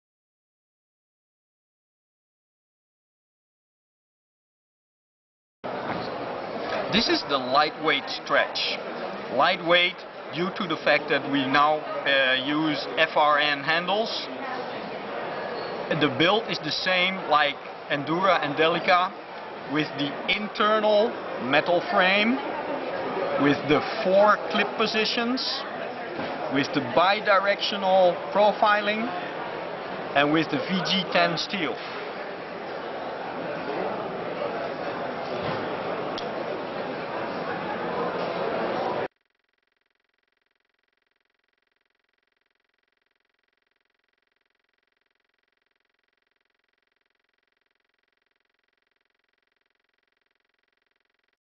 Spyderco Stretch 2 VG10 Prototype
This is the production prototype model Stretch 2 VG10, by Spyderco. This clip was shot on the IWA Show 2009 in Germany.